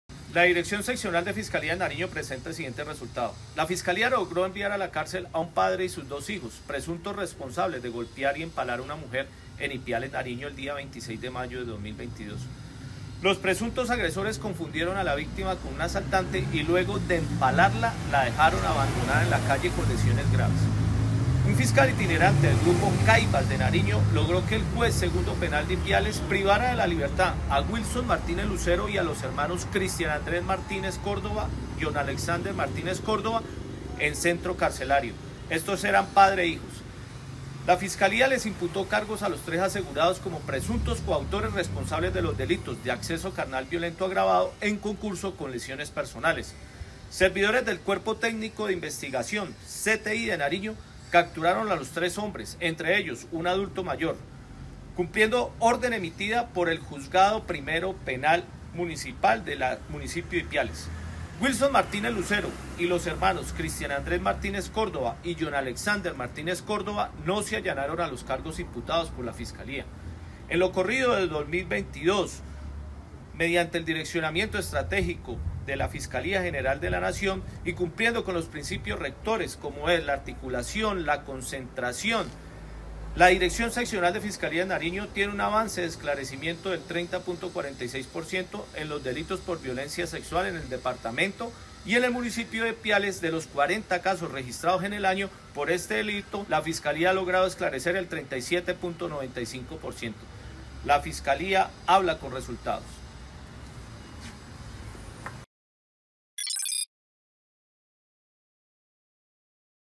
A la cárcel por empalamiento a una mujer en Ipiales
Luego de avalar los argumentos de la Fiscalía, un juez dictó medida de aseguramiento en cárcel a tres hombres, padre e hijos, que habrían sometido a agresiones físicas y sexuales a una mujer en Ipiales, Nariño. El atroz hecho ocurrió este 1 de mayo.